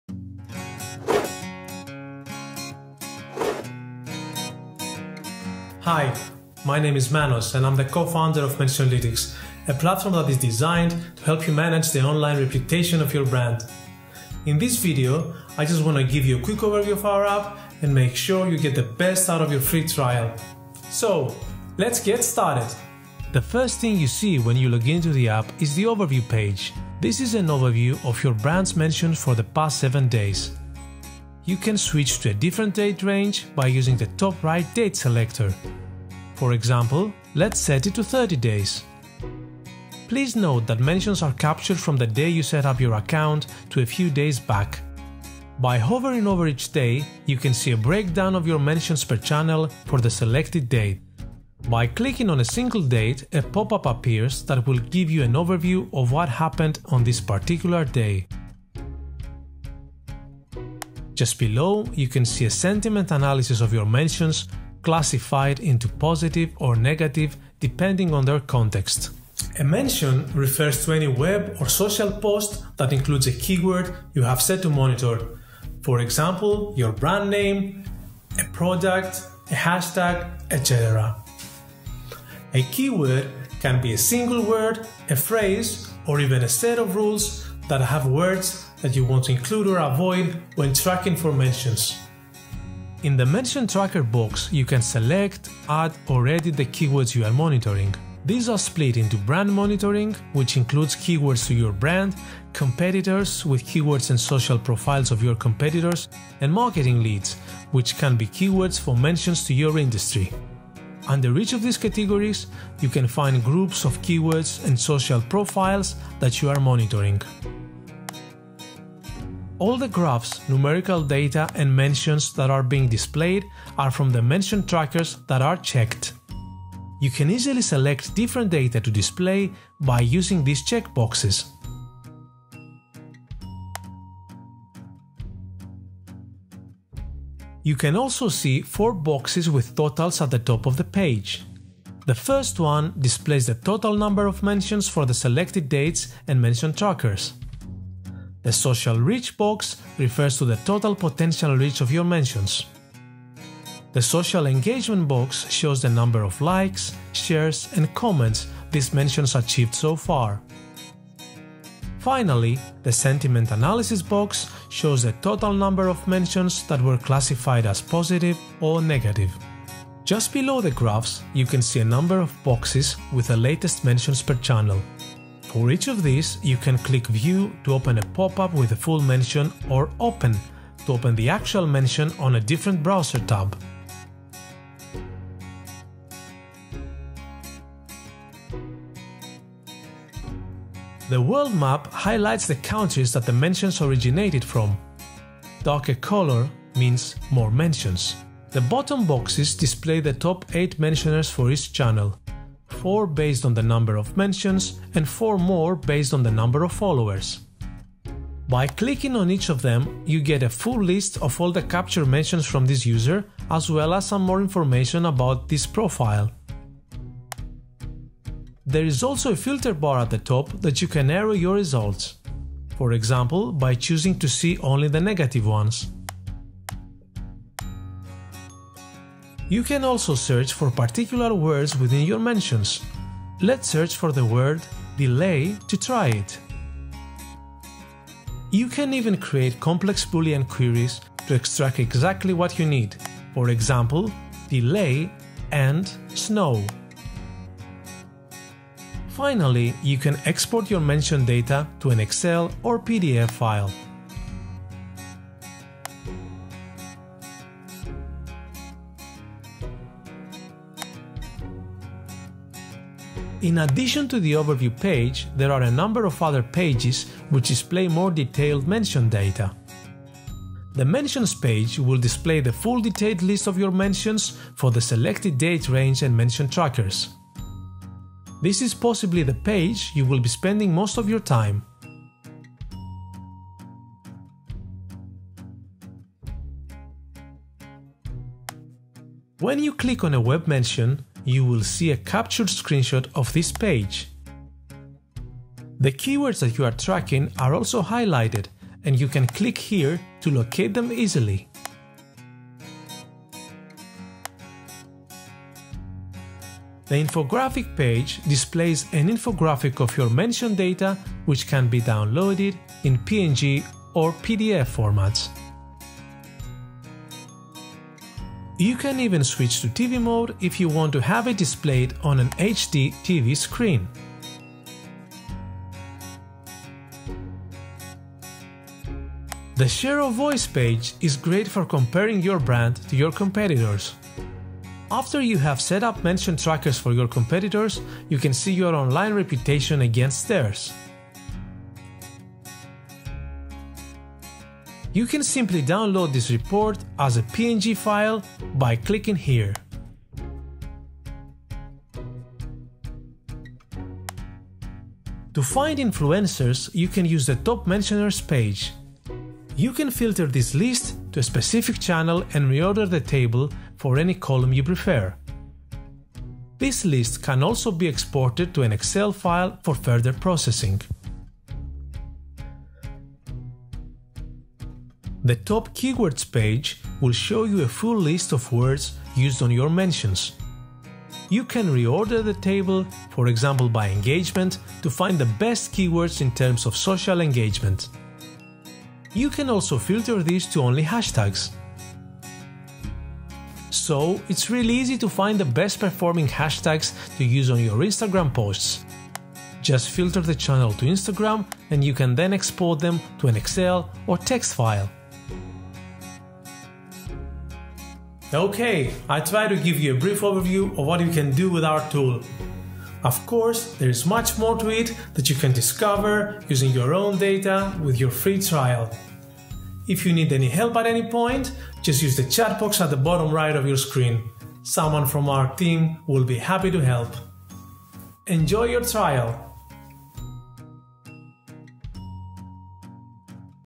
Social Monitoring & Reputation Management with Mentionlytics - Free Trial Guide
This is a quick guide to Social Listening and Reputation Management with Mentionlytics, a powerful Web & Social Media monitoring tool.